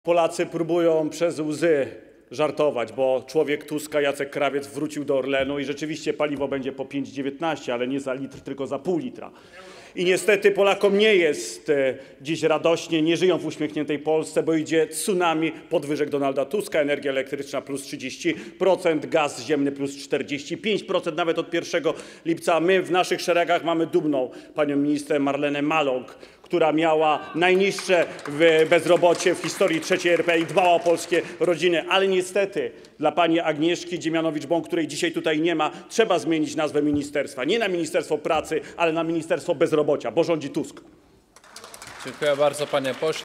Kowalski: idzie tsunami podwyżek Donalda Tuska, to nie jest uśmiechnięta Polska!
11. posiedzenie Sejmu - dzień drugi. 9 maja 2024 r.